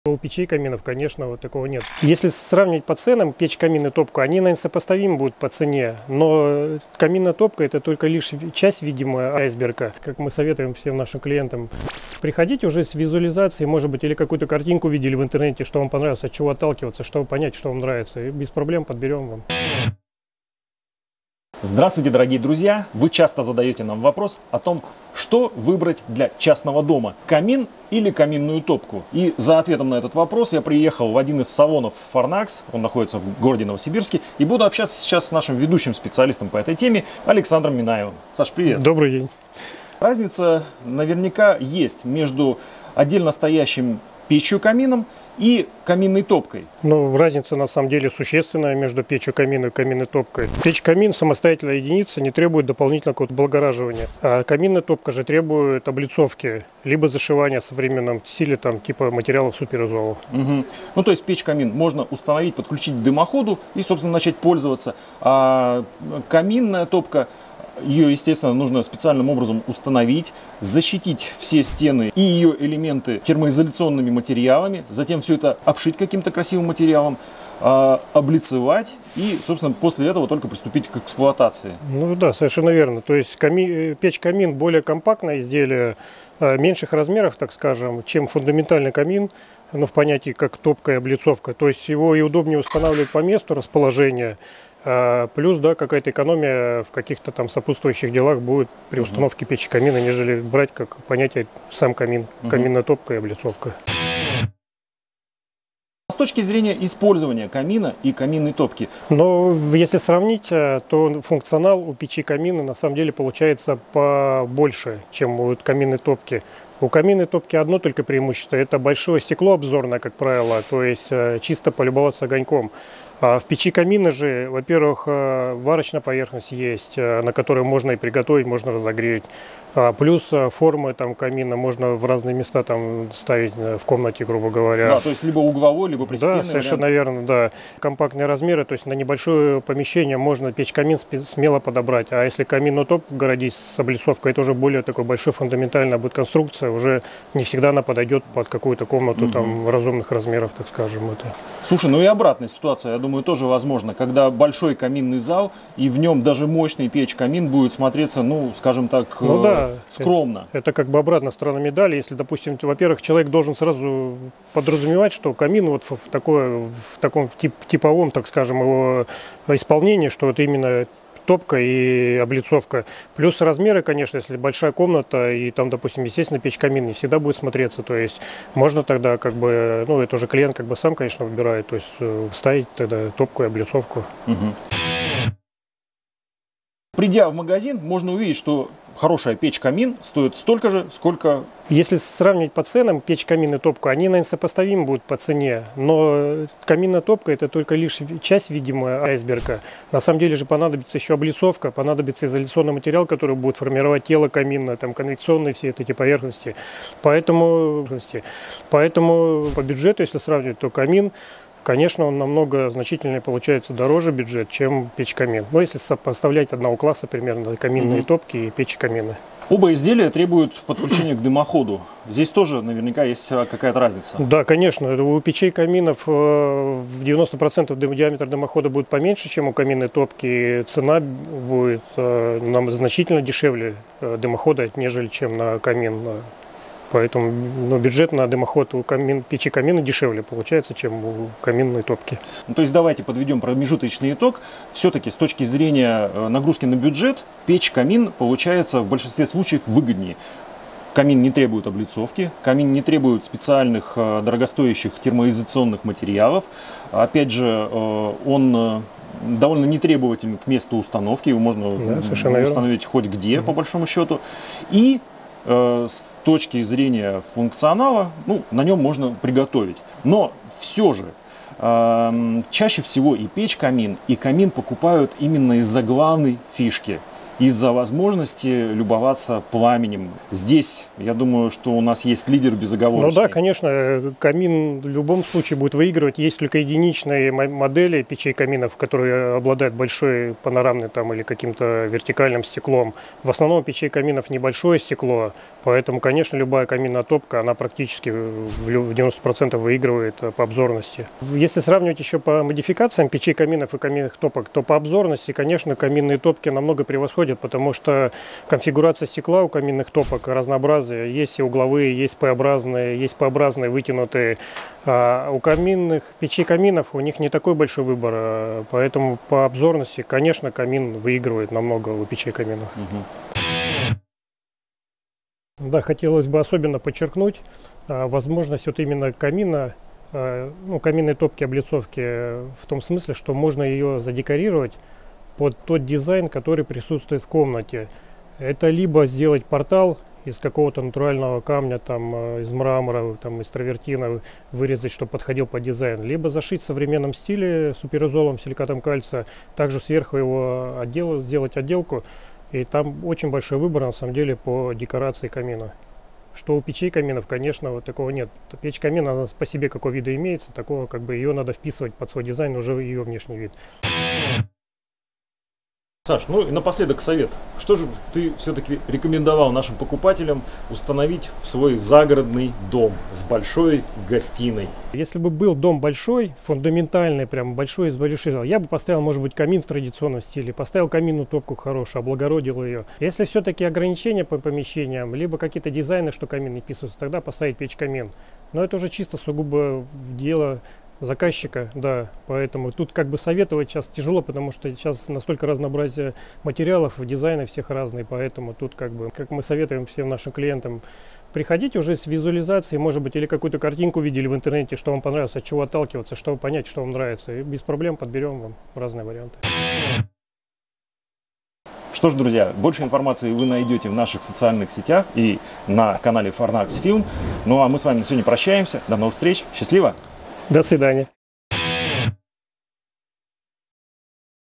Что выбрать печь или камин?! Сравнительный обзор, который поможет сделать правильный выбор.
Многие владельцы частных домов, хотят разместить в своем помещении отопительное оборудование. Такой очаг позволяет не только сделать атмосферу максимально уютной и комфортной, но и обзавестись источником приносящим тепло.
Ещё один вопрос, который возникает в данном случае – это что лучше выбрать печь или камин? На этот вопрос нельзя найти однозначного ответа, так как и у того и другого агрегата есть свои исключительные особенности и преимущества. Однако сделать выбор возможно. Для этого мы сняли сравнительный обзор , который поможет вам сделать правильный выбор, мы предлагаем вам послушать наших экспертов и посмотреть фото уже готовых установок.

Для тех кто хочет узнать преимущества и особенности быстрее:
4:07 - 4:30 - преимущество печей-каминов
5:05 - 6:00 достоинство каминных топок
6:00 - 6:44 Советы специалиста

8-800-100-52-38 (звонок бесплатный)